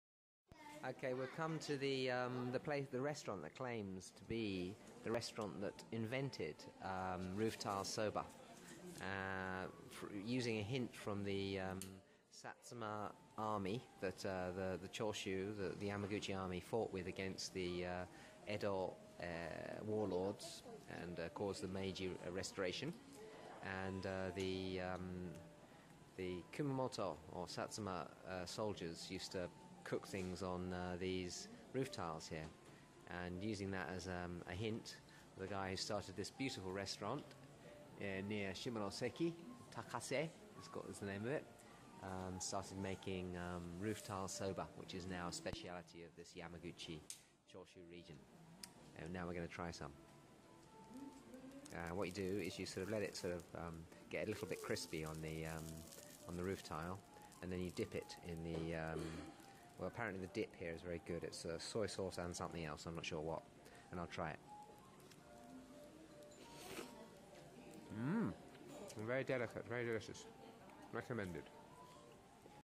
The Origin of Roof Tile Buckwheat Noodles (Stabilized with Sony Vegas)
At the restaurant of the descendent of the man that invented it in Kawatana Spa Westerner Yamaguchi. It is a good restaurant, and the noodles are tasty.

This is the new stabilized version, stabilized with Sony Vegas 12 (about 400 USD) because YouTube no longer offers stabilization. I will leave the old version up for a while too for comparison below.

This is in Yamaguchi Prefecture about an hour's drive from Yamaguchi City. Thank you New York Times!